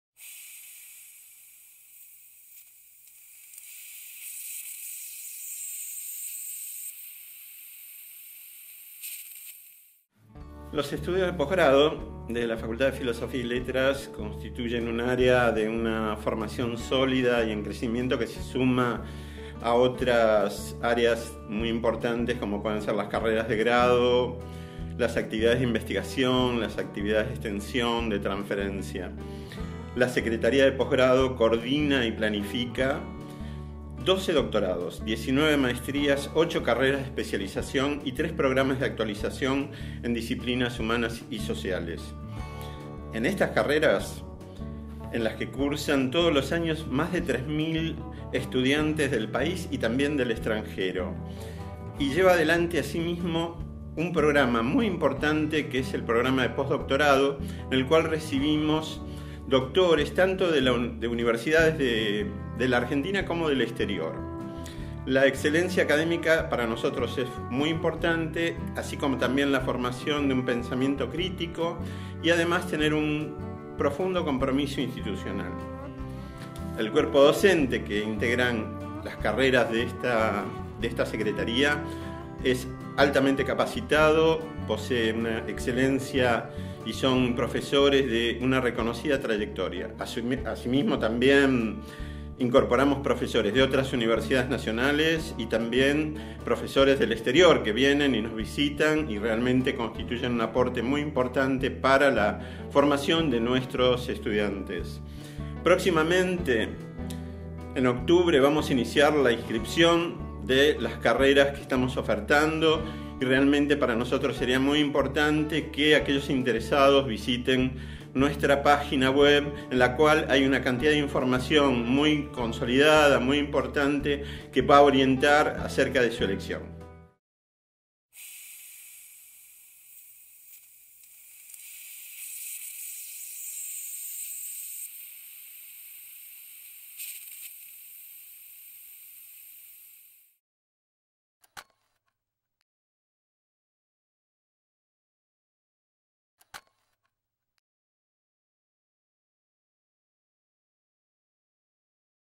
Secretaria de Posgrado
Los estudios de Posgrado de la Facultad de Filosofía y Letras constituyen un área de formación sólida y en crecimiento, que se suma a sus prestigiosas carreras de grado y sus comprometidas actividades de investigación, extensión y transferencia. La Secretaría de Posgrado planifica y coordina 12 doctorados, 19 maestrías, 8 carreras de especialización y 3 programas de actualización en disciplinas humanas y sociales, carreras en las que cursan todos los años unos 2000 estudiantes del país y el extranjero. Lleva adelante, asimismo, un programa de posdoctorado, e incluye entre sus actividades conferencias, charlas y cursos. La excelencia académica, el pensamiento crítico y el compromiso institucional caracterizan a las autoridades de las distintas carreras. El cuerpo docente está integrado por docentes e investigadores de la Facultad y por docentes e investigadores invitados, todos ellos destacados en sus diferentes especialidades.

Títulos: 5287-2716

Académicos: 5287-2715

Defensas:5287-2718